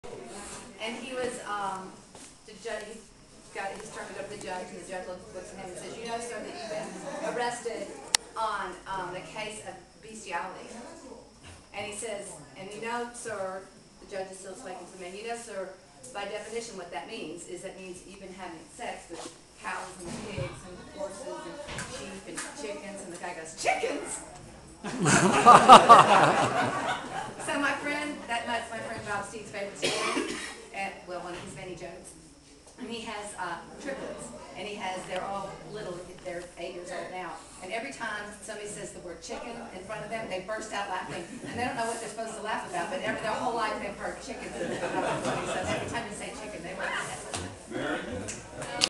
Chicken!
KK tells the chicken joke.